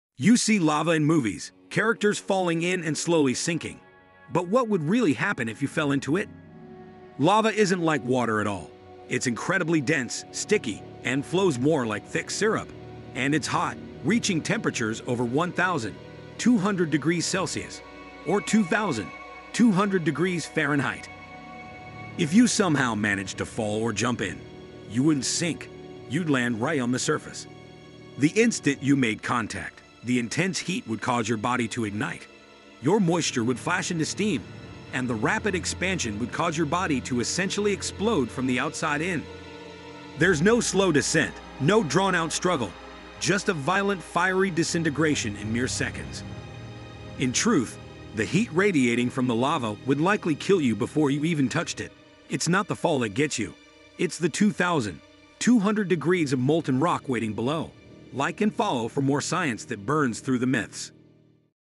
If You Fell Into Lava — The Terrifying Science Explained
What actually happens if you fall into lava?
Spoiler: It’s not like the movies. You won’t sink — you’ll explode.

In this video, we break down the brutal science behind what lava does to the human body, from the temperature of molten rock to the physics of your last seconds. It’s fast. It’s violent. And it's 100% real science.